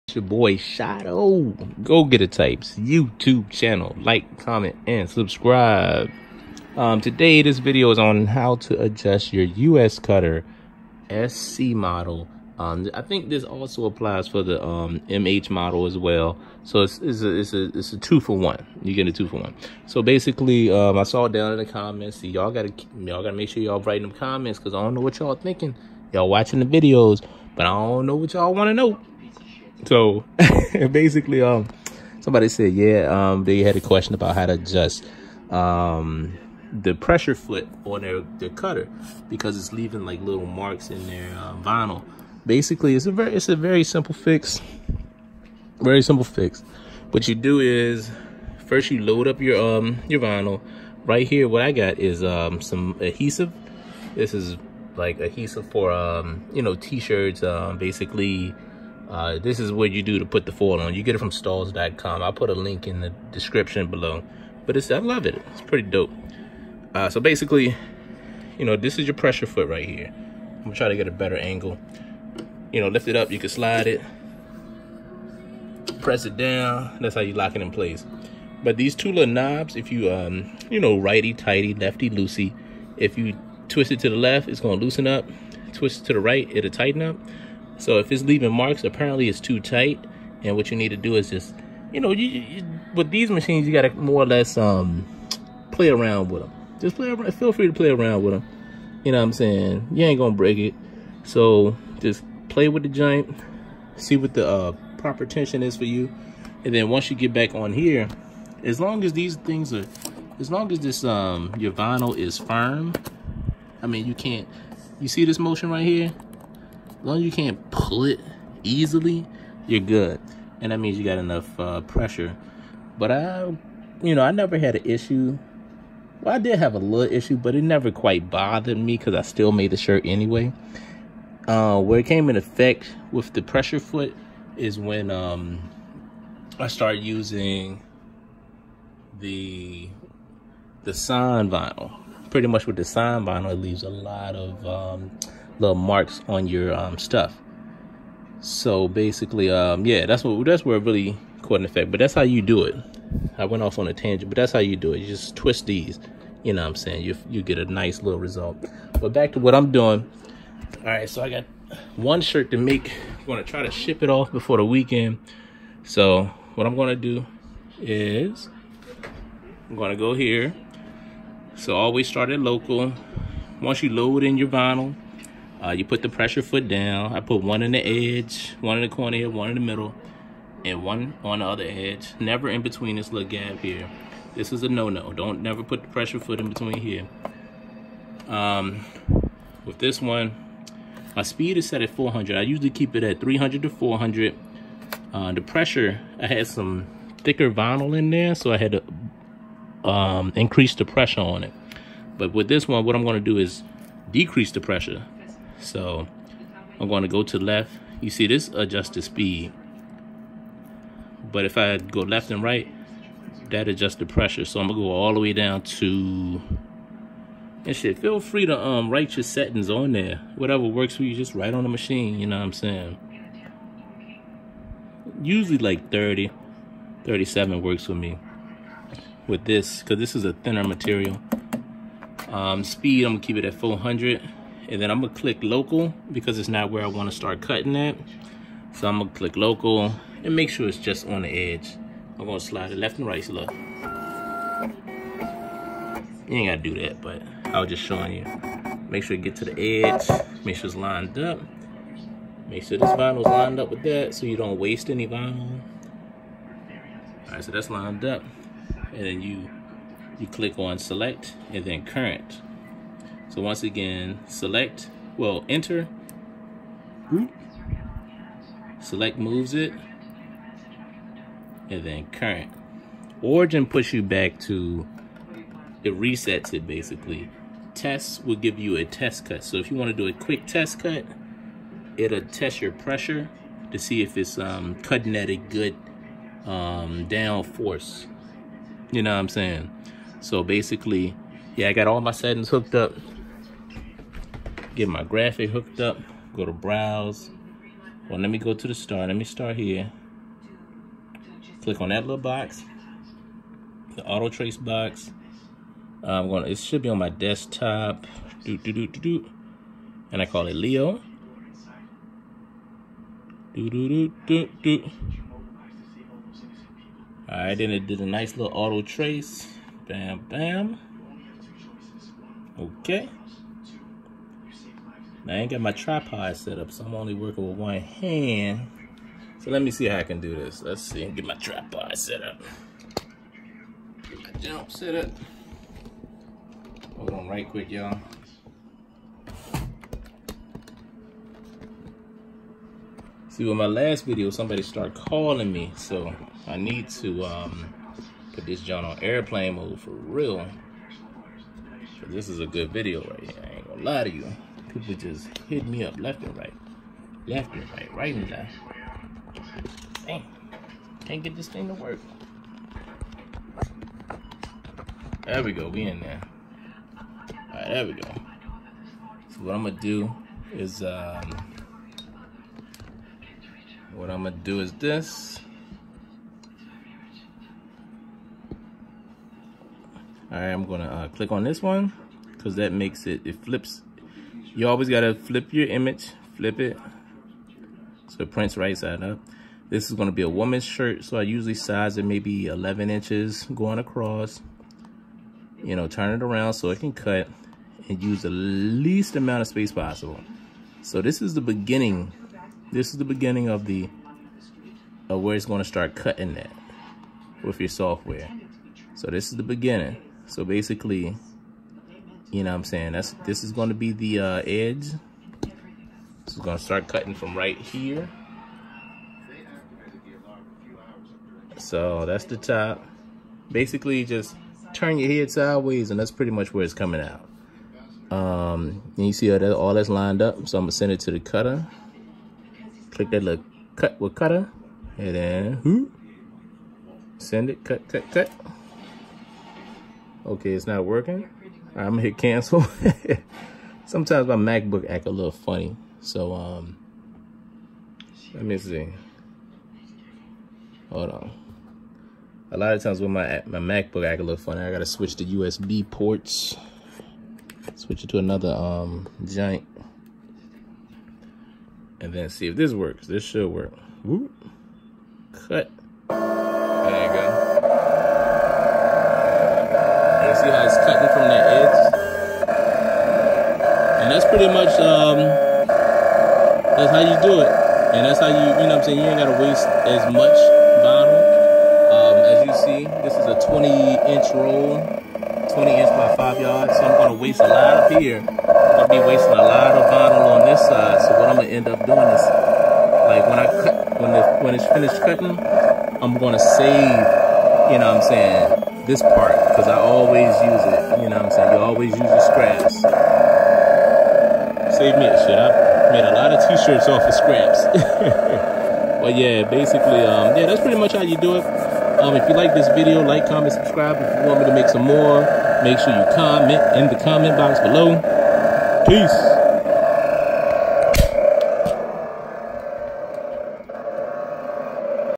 How to use uscutter sc2 uscutter mh vinyl cutter
This video shows you the basics on how to load vinyl in the uscutter sc model and how to adjust the pressure foot so that you dont have marks in you vinyl. Also this video will show you how to save vinyl.